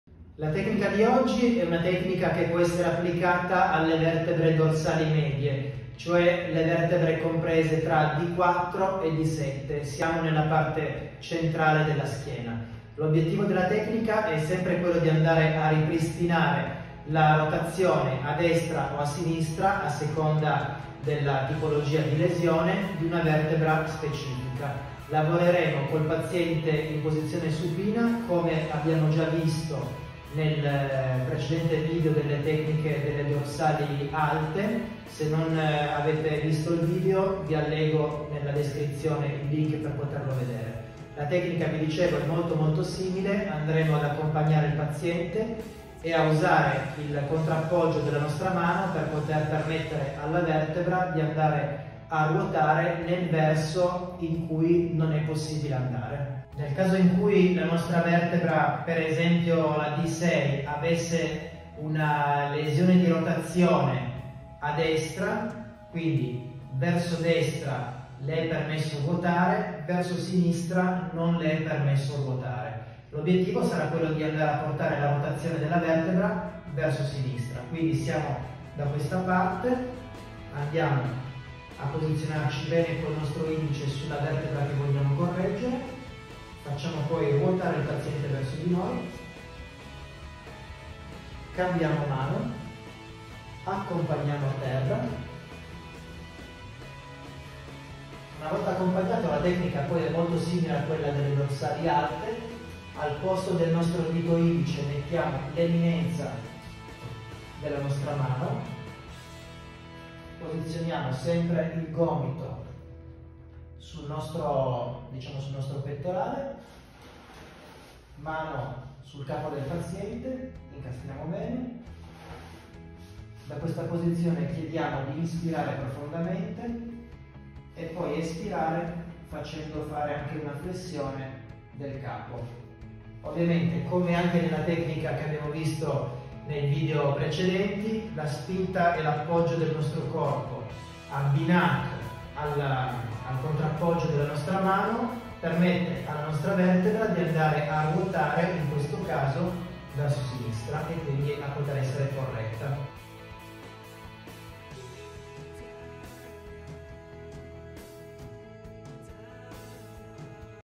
Manipolazione vertebre dorsali “medie”🦴💥
Come trattare il mal di schiena quando localizzato nella parte centrale della schiena?🤔

Questa tecnica è molto efficace e ha il vantaggio di poter essere utilizzata in quasi tutti i pazienti! 👨🏼‍⚕️

Se hai perso il video relativo alle vertebre dorsali “alte”, ecco il link ⬇️